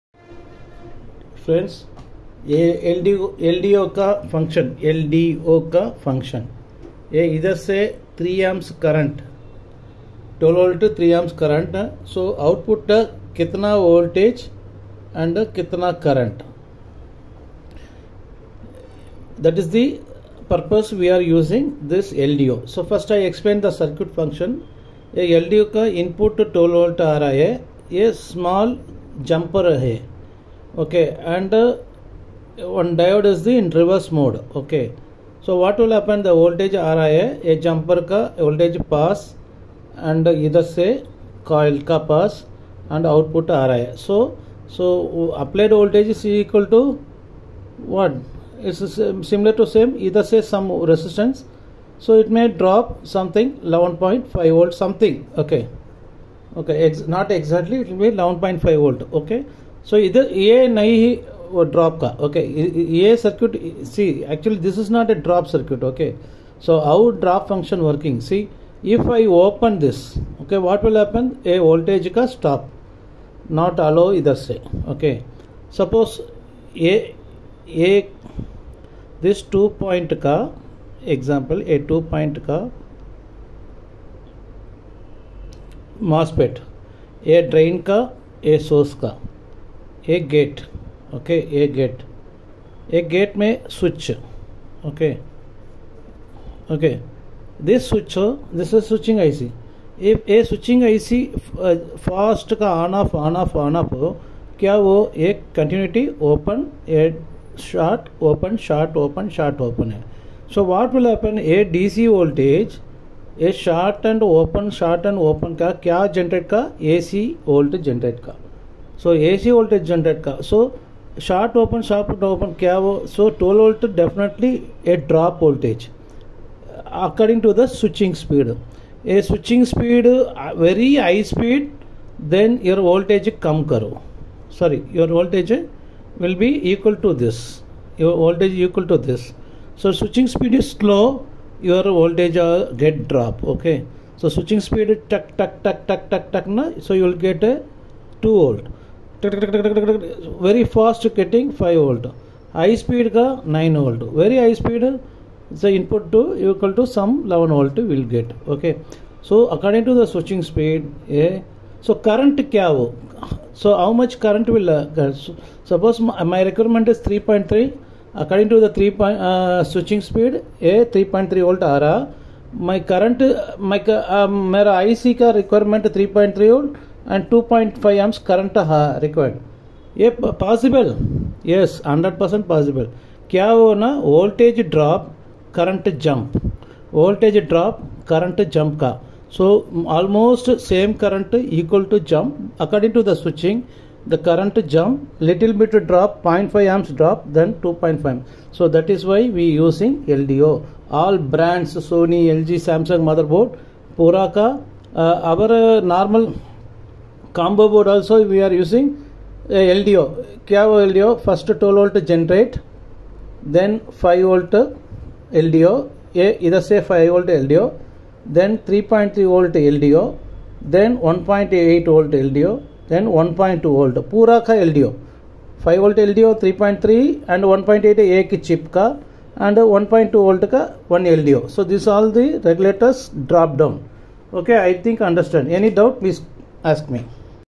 LCD LED TV - What is LDO/ DC to DC Buck and How it works
Full detailed Circuit explained in simple diagram